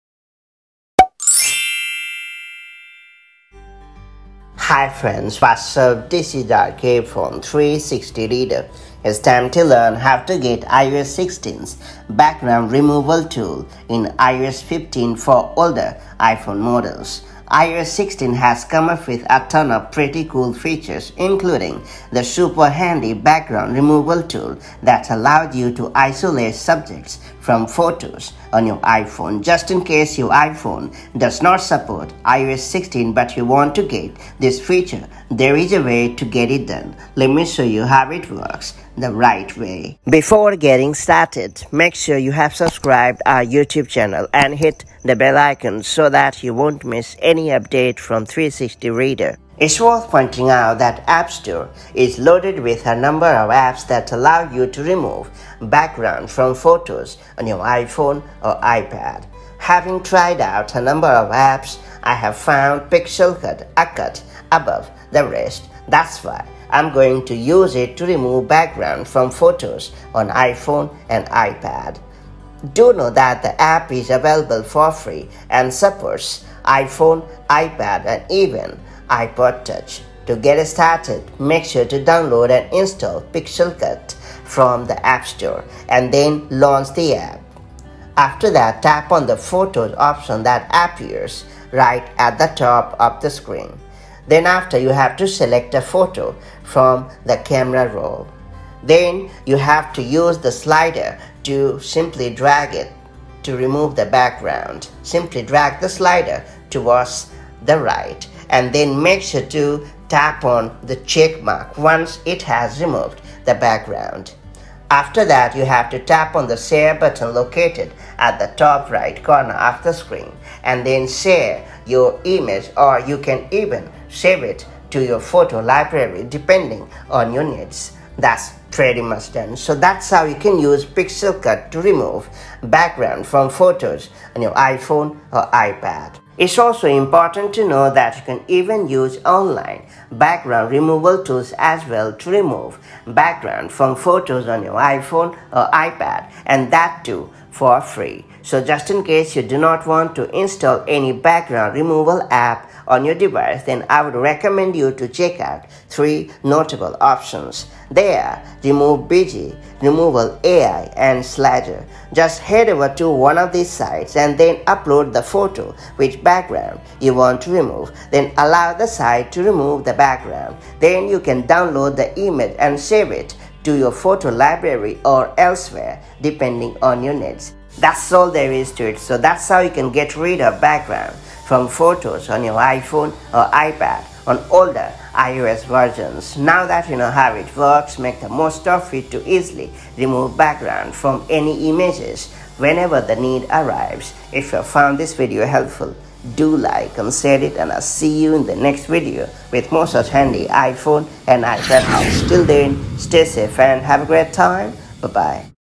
How to Get iOS 16’s Background Removal Tool in iOS 15 for Older iPhones 🔥🔥
Here is how you can get iOS 16’s background removal tool in iOS 15 for older iPhone and iPad models. Let’s learn how it’s done!